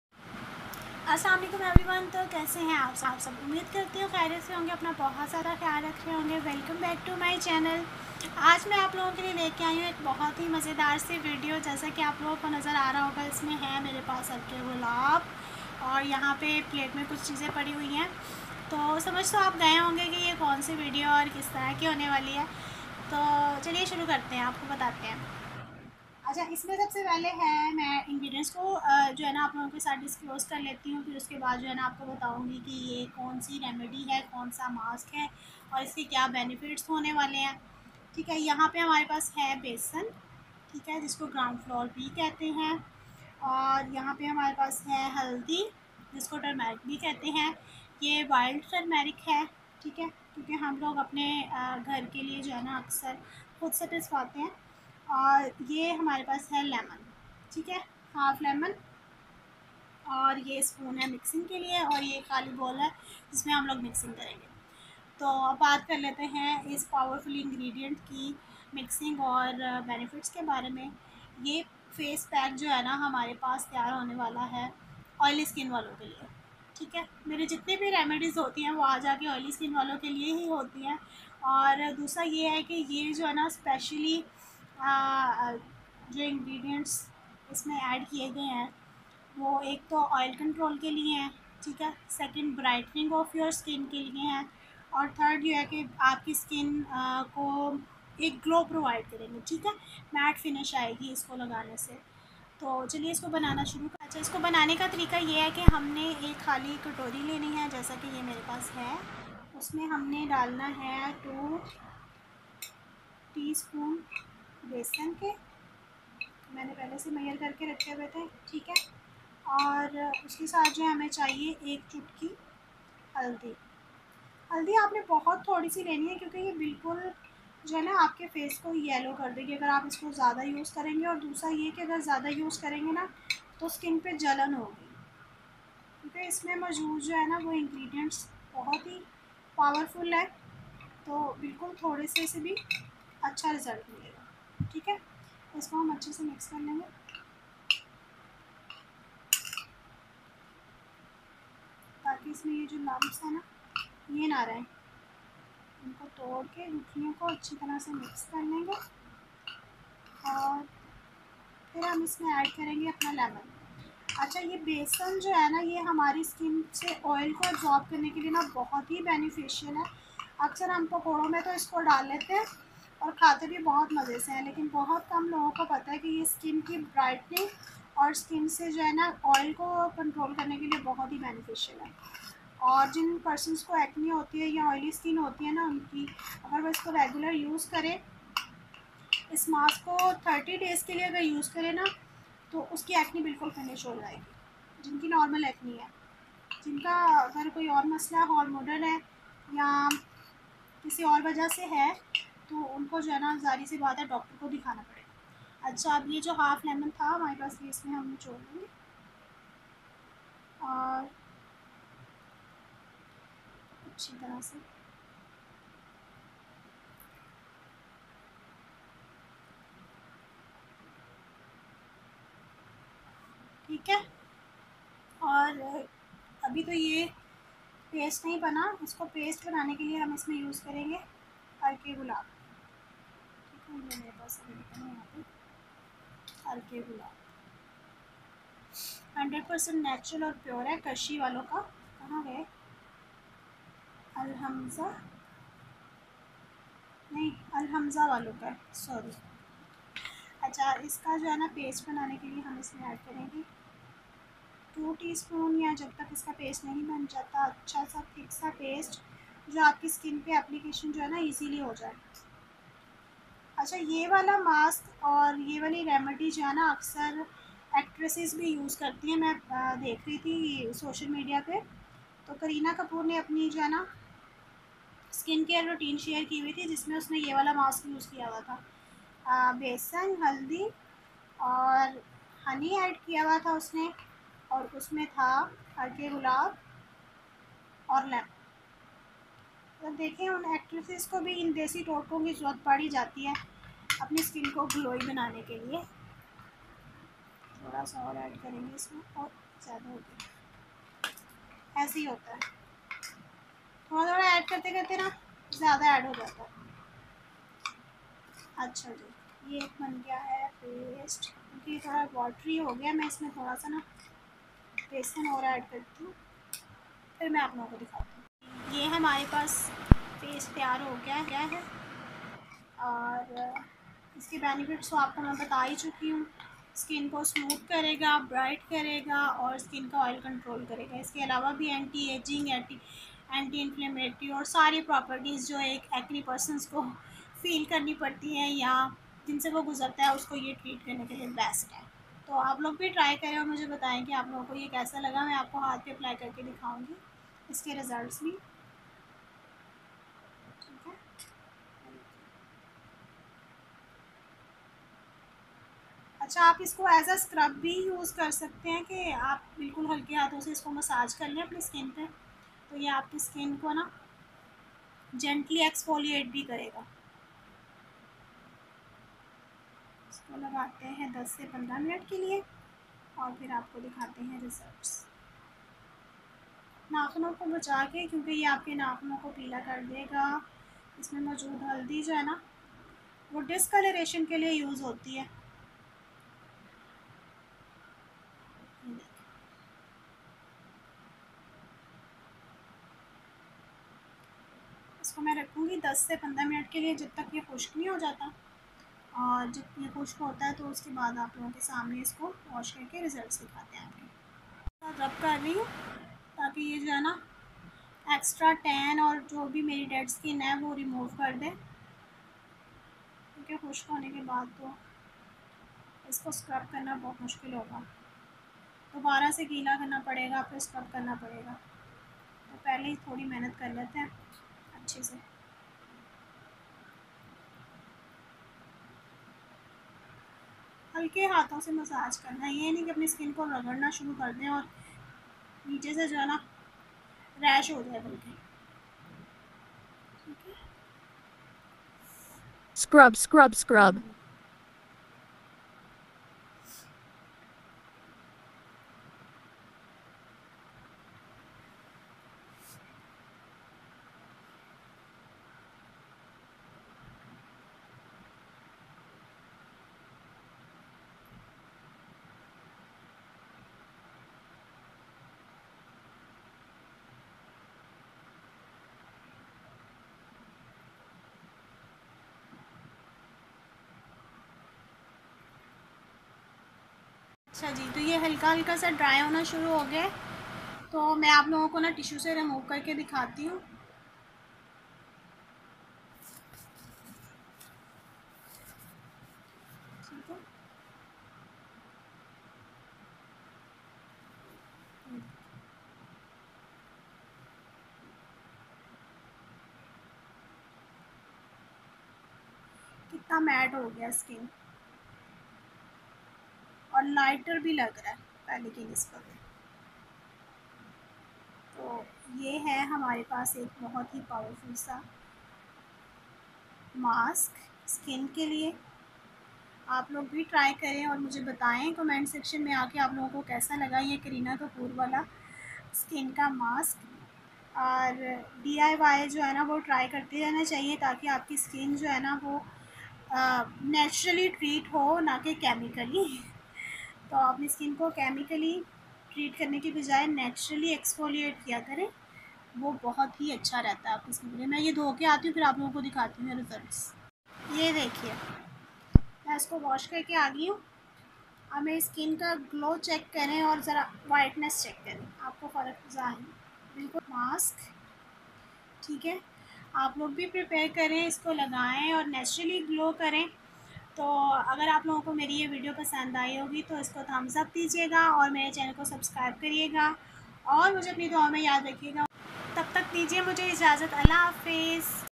kareen kapoor skin care mask for oily skin | DIY by Zohra's
gram flour
turmeric
lemon
rose water